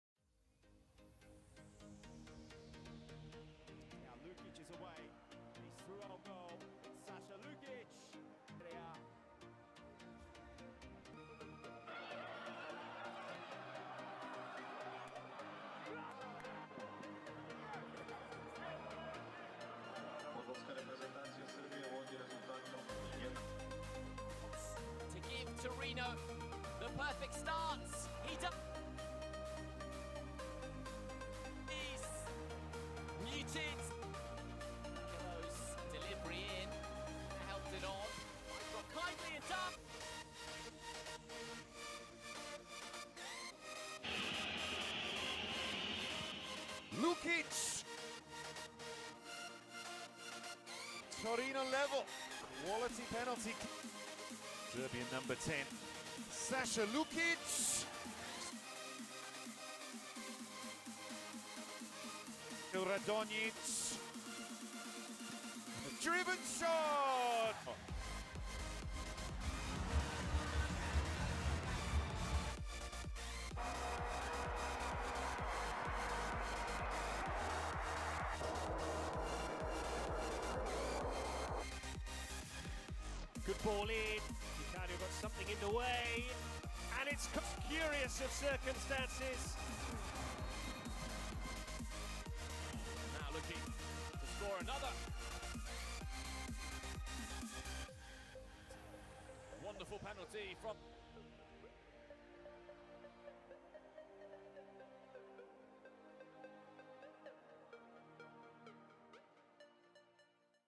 Saša Lukić - All his goals in 2021 and 2022!
Fulham player now!

Torino was his club and accepted proposal on €10m fee.

He is from Serbia.

So in Premier League now!
How will he do?

Watch all his goals from last two years here!

How to watch PL in your country?